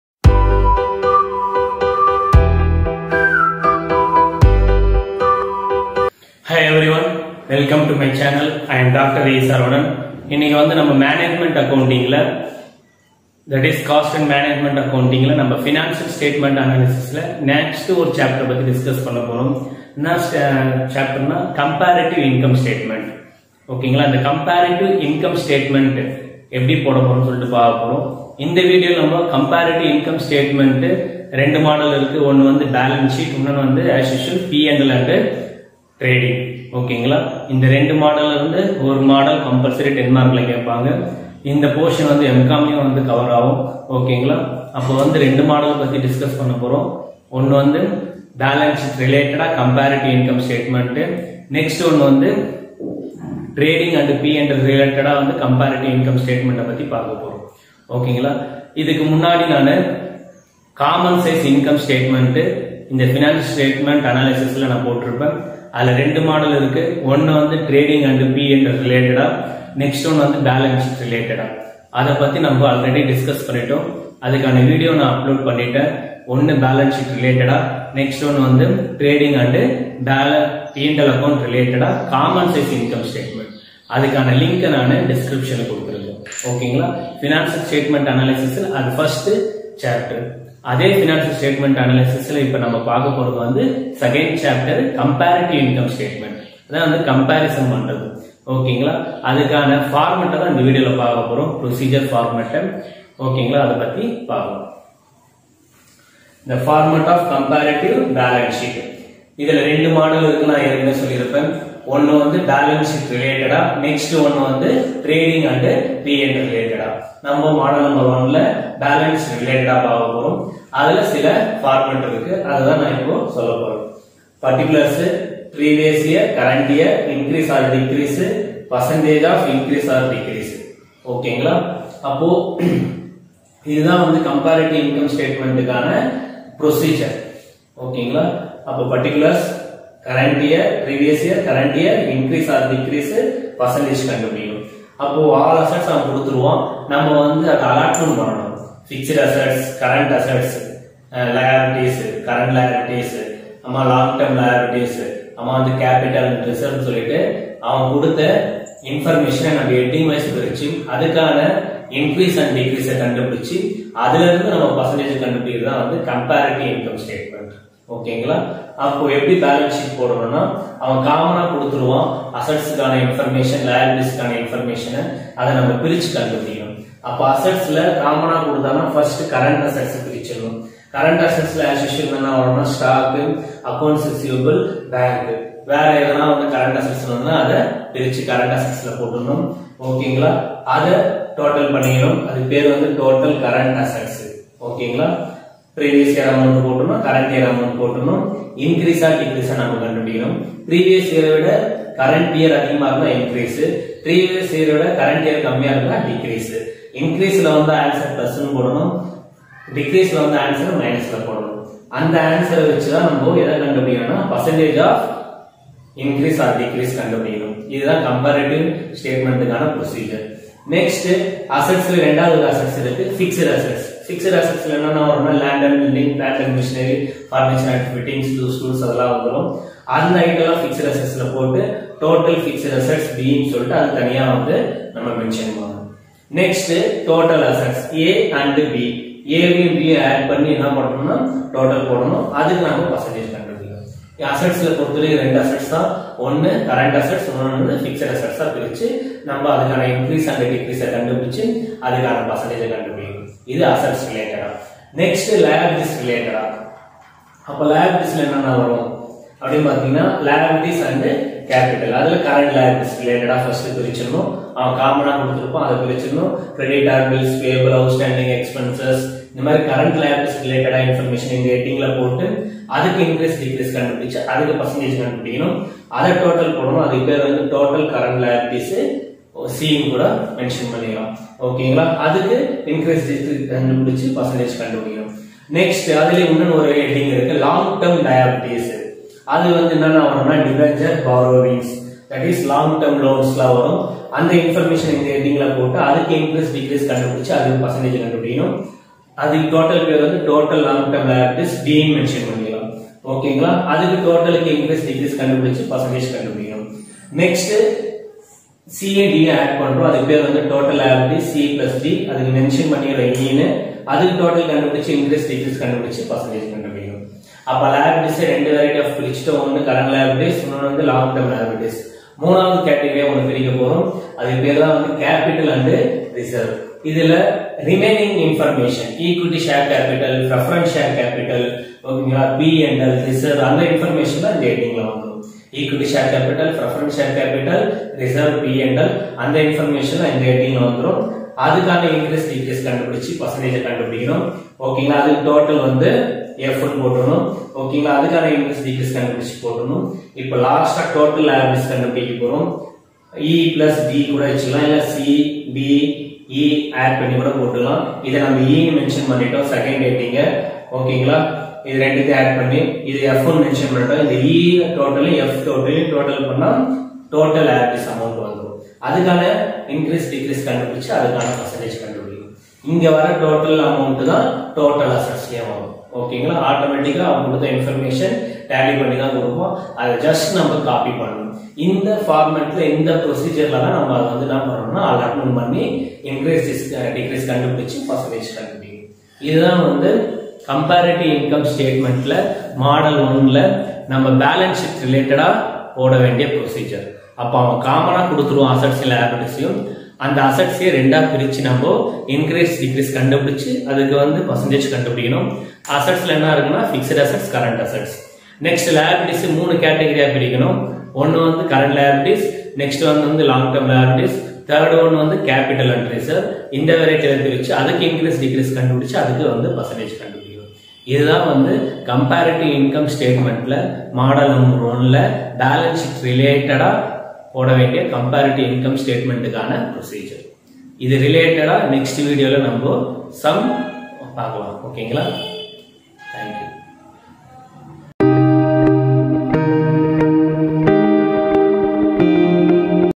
COST AND MANAGEMENT ACCOUNTING | FINANCIAL STATEMENT ANALYSIS | COMPARATIVE INCOME STATEMENT- FORMAT
Common to B.Com and M.Com, in this video you will learn about format of Comparative Income Statement.